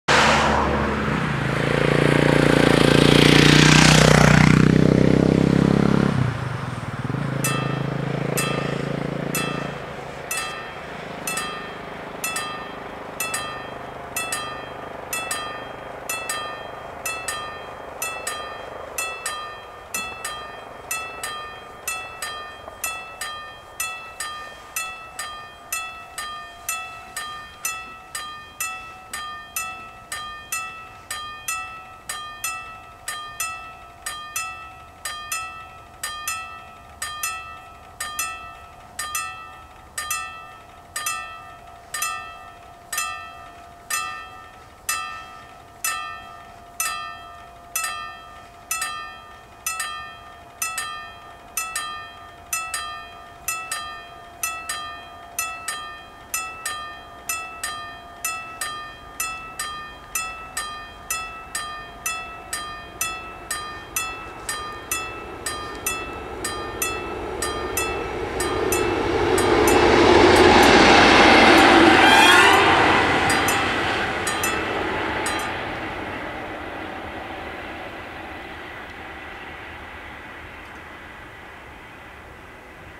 Spoorwegovergang Hamm (D) // Railroad crossing // Bahnübergang
Locatie: Wilhelm-Lange-Straße
Traject: Hamm - Unna
Rode lichten: 4
Ontruimingslichten: 2
Bellen: 2
Bomen: 2
Andreaskruisen: 4
Passeren:
- BR 291 richting Hamm